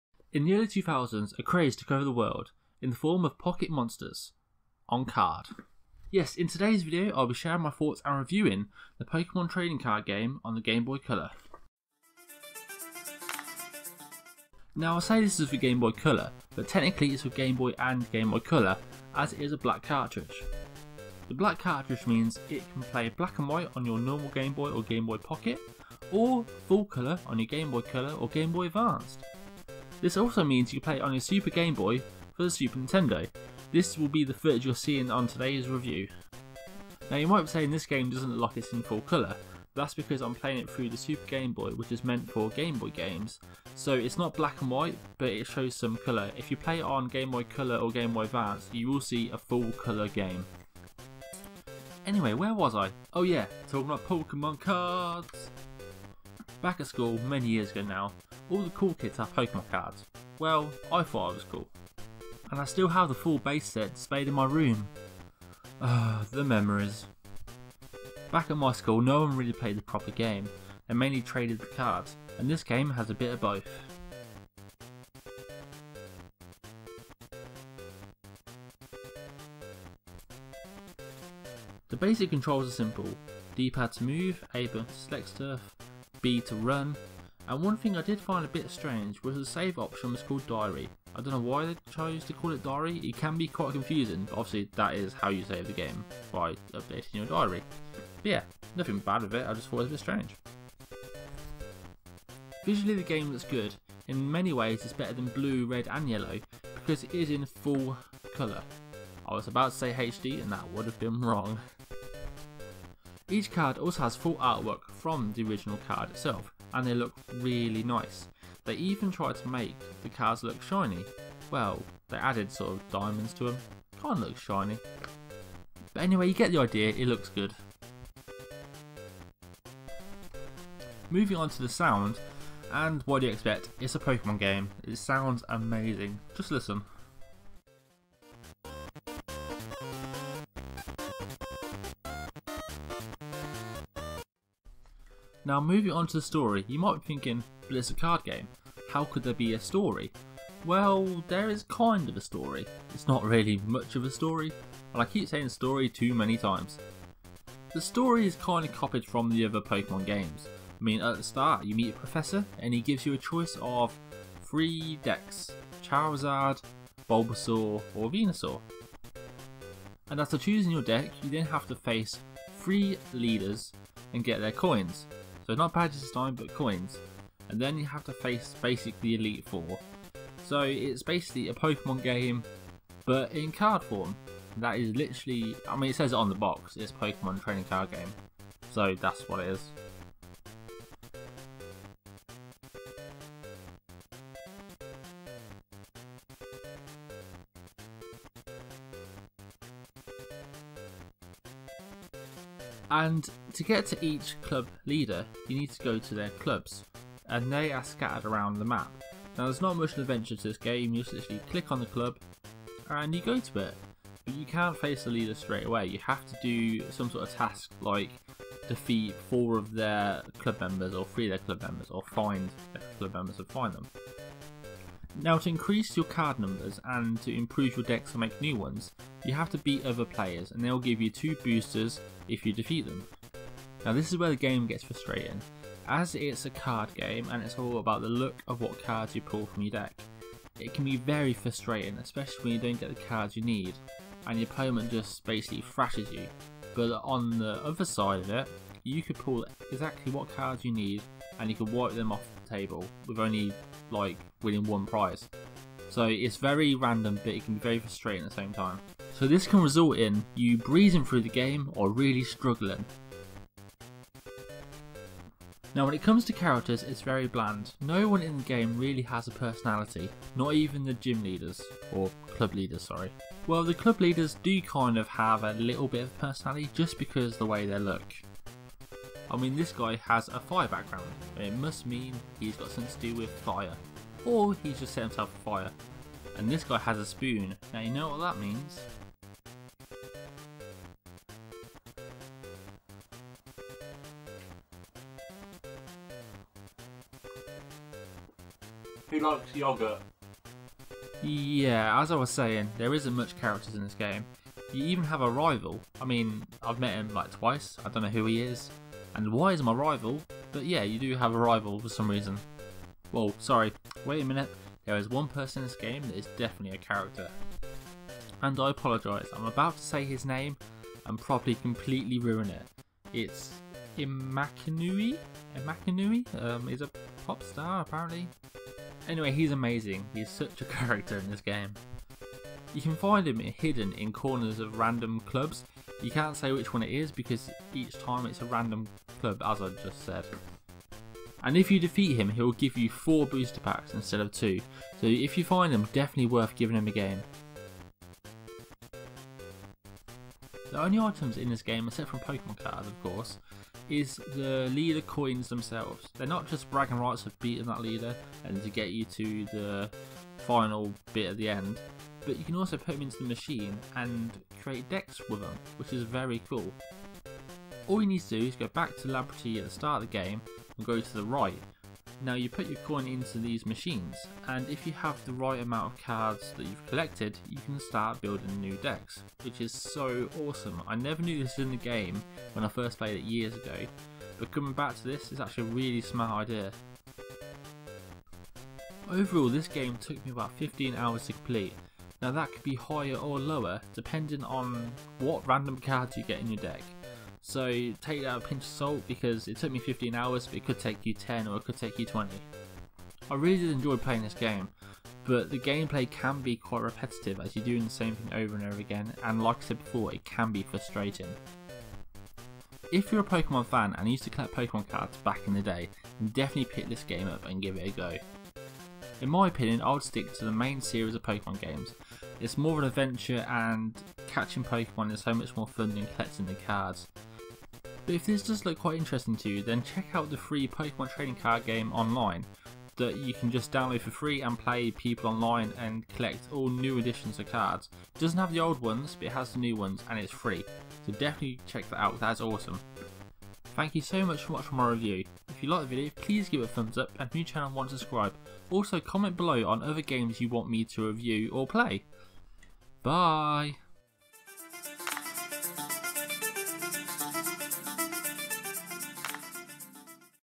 Pokemon Trading Card Game Review (GB)
Today I am reviewing Pokemon Trading Card Game for the Nintendo Game Boy Colour.

If you like the video please remember to give it a thumbs up and why not subscribe to my channel as well as follow me on all the social pages listed below:

Just want to say thank you to Just Kiddin for letting me use a bit of the music for this.